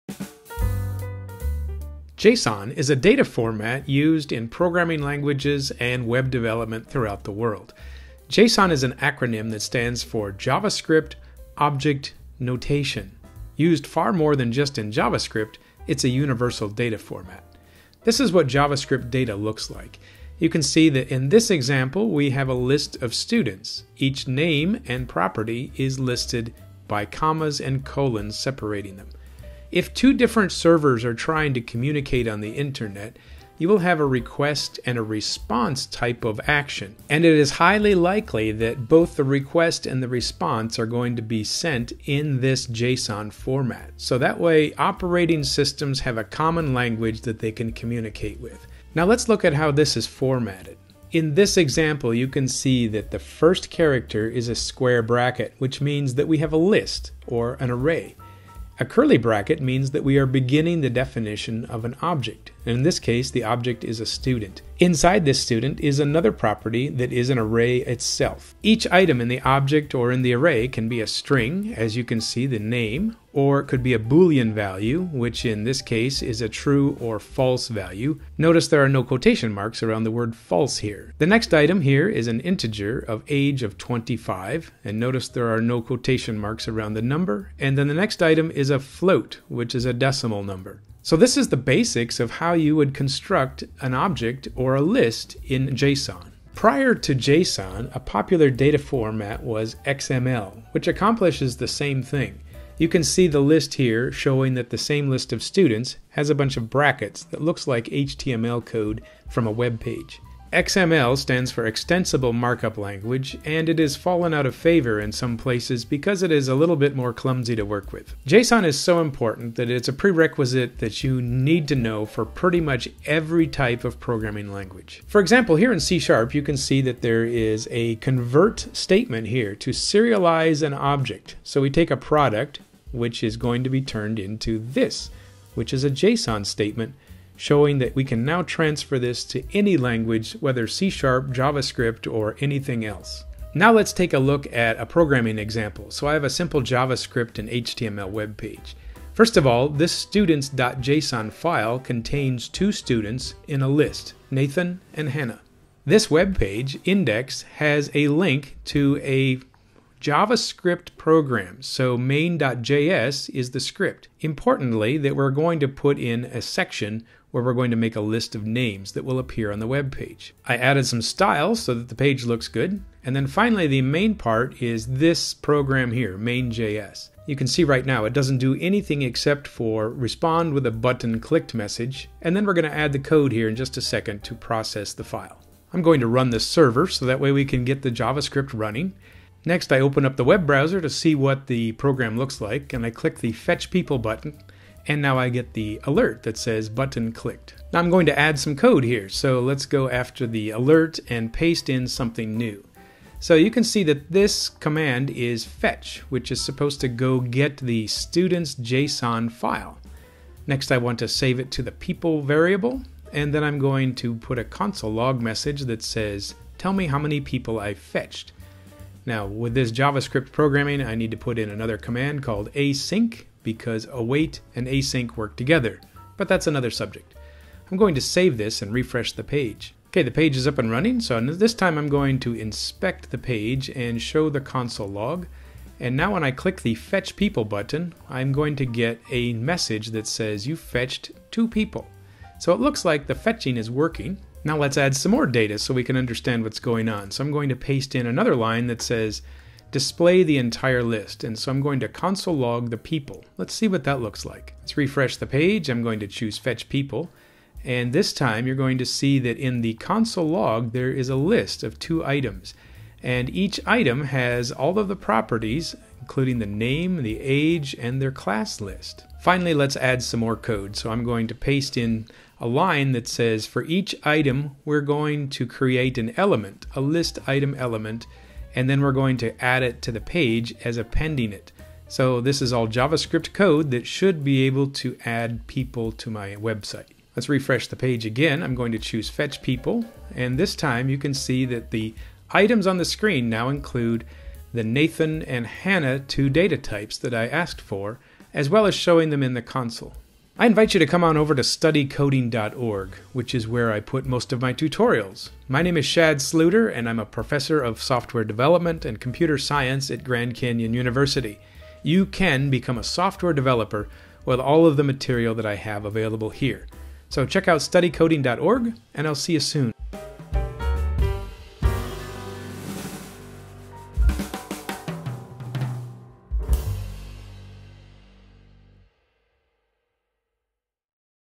Learn JSON in 5 minutes
Learn how to format, parse and use JSON data in a JavaScript application.